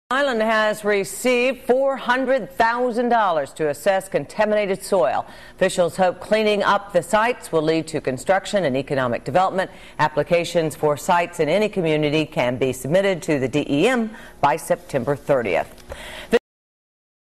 RI soil contamination check
Rhode Island will receive money to assess contaminated soil